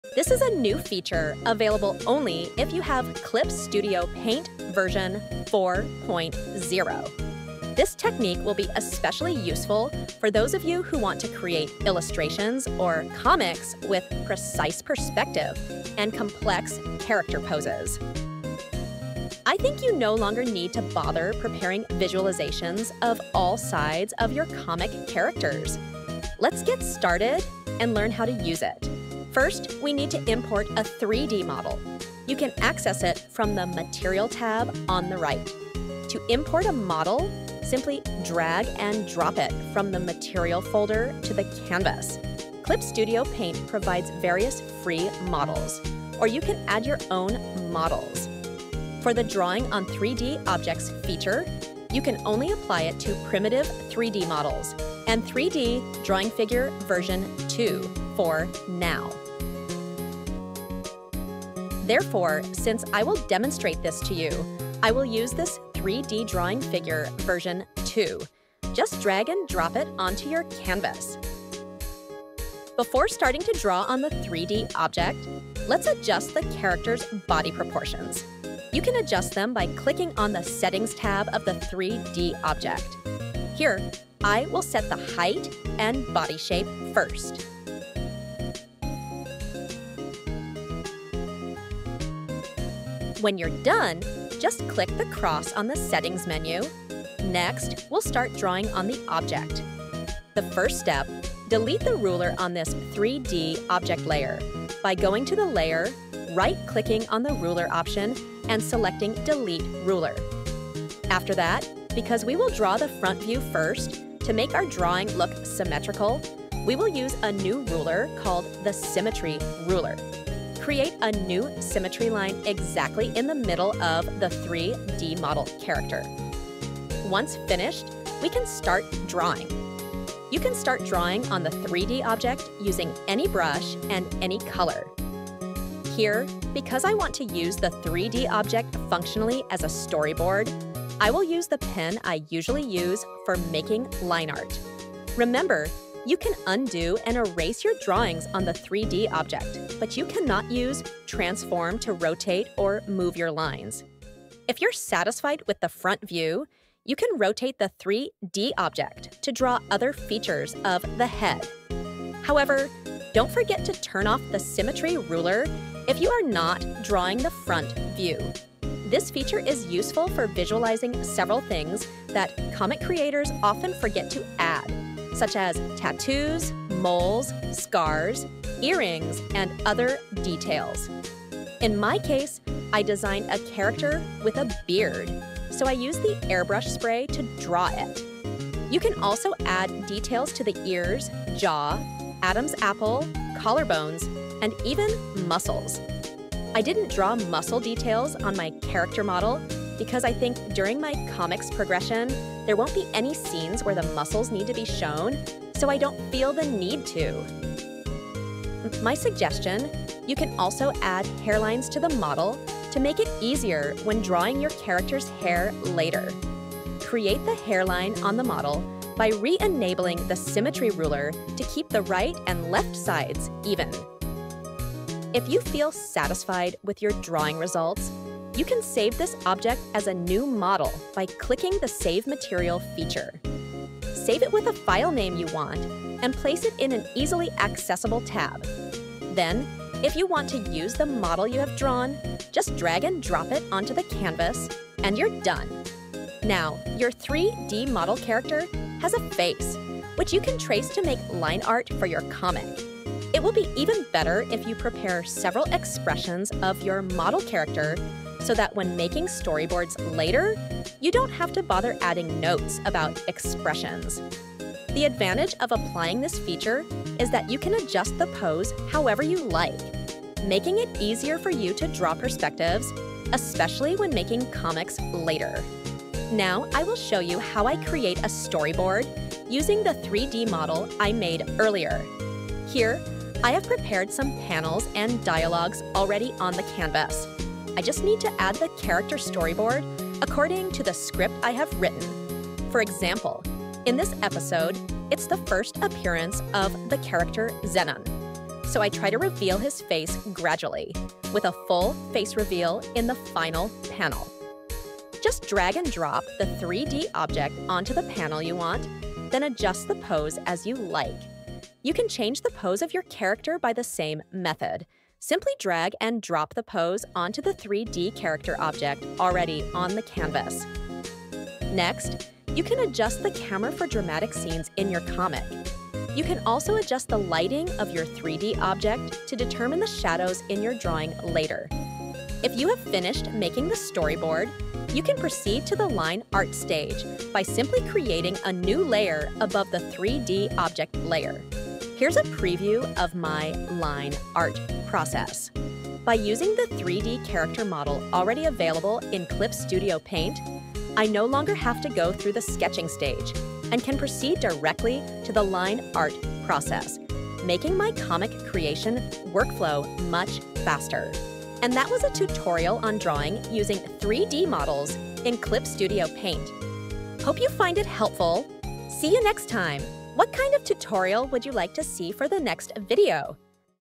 Clip Studio Paint 4.0 Feature to Make Your Storyboard Easier (Webtoon Making #10)
It's even better if you prepare custom expression for your character model too.
00:00 preview
00:28 Importing 3D model to your canvas
01:13 Adjusting 3D model body proportions
01:39 Drawing on 3D model
04:18 Making Line art from 3D model
04:42 Storyboard creation progress
06:00 Webtoon